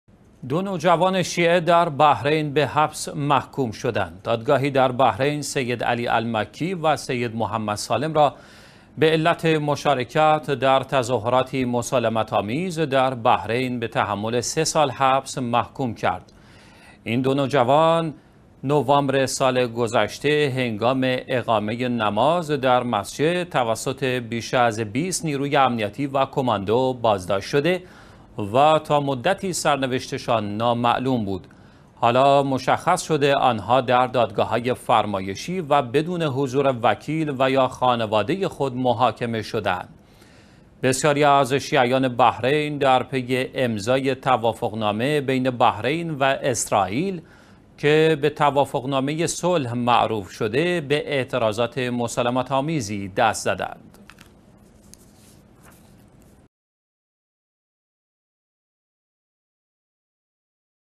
جریمه حبس برای دو جوان شیعه بحرینی بدلیل حضور در تظاهرات
دو نوجوان شیعه در بحرین به حبس محکوم شدند.
دادگاهی در بحرین سید علی المکی و سید محمد سالم را به علت مشارکت در تظاهراتی مسالمت آمیز در بحرین به تحمل ۳ سال حبس محکوم کرد.
این دو جوان شیعه نوامبر سال گذشته هنگام اقامه نماز در مسجد توسط بیش از ۲۰ نیروی امنیتی و کماندو بازداشت شده و تا مدتی سرنوشتشان نامعلوم بود.
حالا مشخص شده آنها در دادگاه های فرمایشی و بدون حضور وکیل و یا خانواده خود محاکمه شده اند.
بسیاری از شیعیان بحرین در پی امضای توافق نامه بین بحرین و اسرائیل که به توافق نامه صلح معروف شده به اعتراضات مسالمت آمیزی دست زدند.